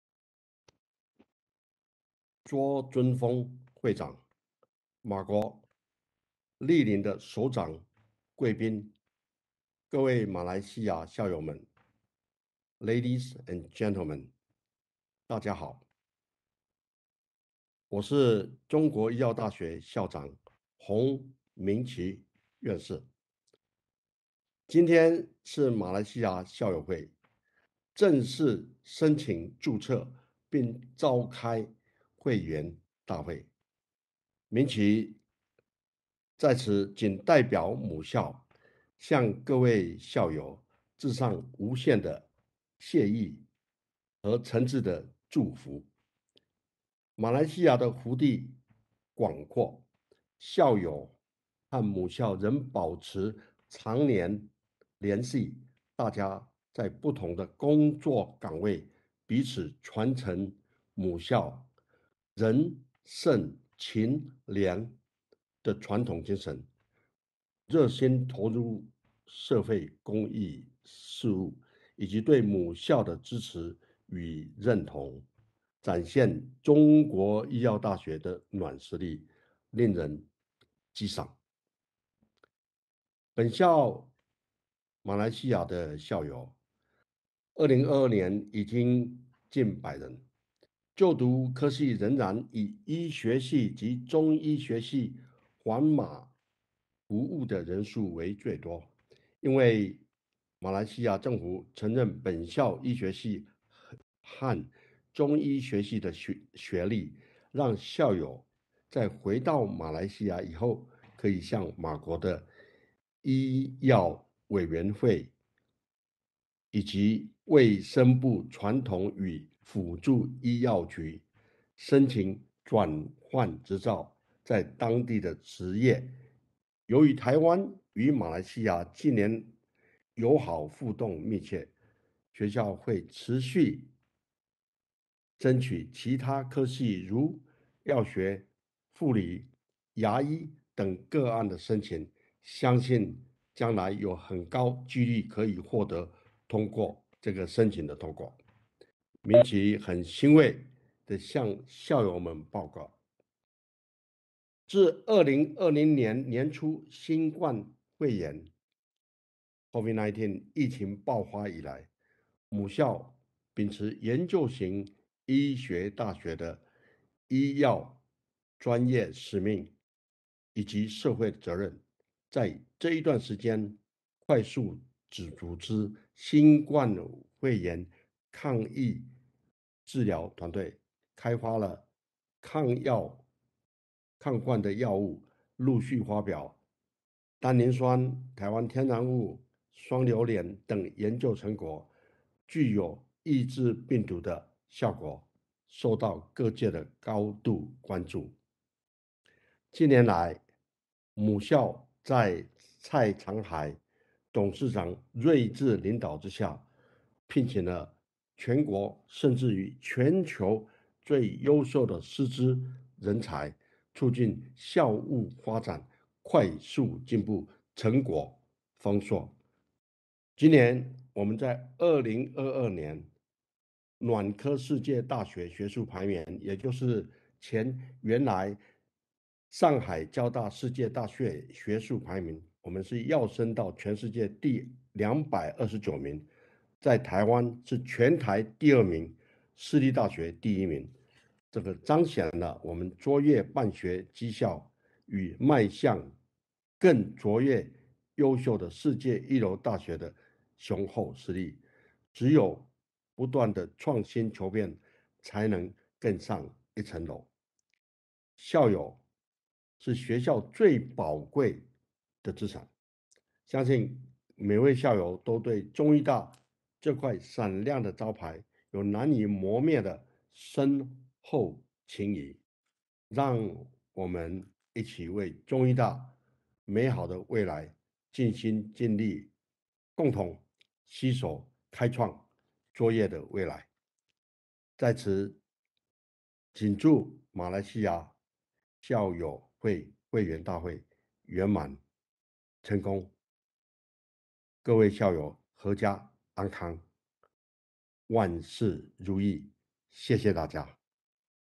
中國醫藥大學馬來西亞校友會正式註冊成立大會
2022年10月7日馬來西亞校友會正式註冊成立大會
校長洪明奇院士賀詞